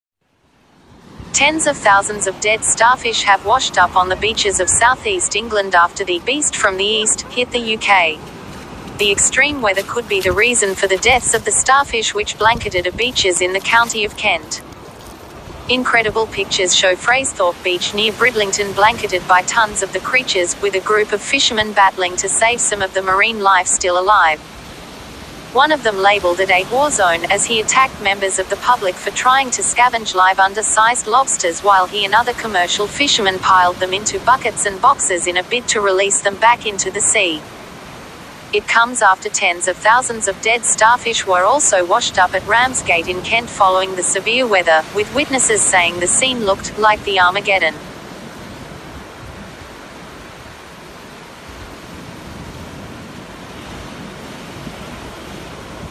Thousands of dead starfish wash up on UK beaches following the 'Beast from the east'
Tens of thousands of dead starfish have washed up on the beaches of southeast England after the "Beast from the East" hit the UK. The extreme weather could be the reason for the deaths of the starfish which blanketed a beach's in the county of Kent.

Incredible pictures show Fraisthorpe beach near Bridlington blanketed by tonnes of the creatures, with a group of fishermen battling to save some of the marine life still alive.

One of them labelled it a 'war zone' as he attacked members of the public for trying to scavenge live under-sized lobsters while he and other commercial fisherman piled them into buckets and boxes in a bid to release them back into the sea.

It comes after tens of thousands of dead starfish were also washed up at Ramsgate in Kent following the severe weather, with witnesses saying the scene looked 'like the armageddon'.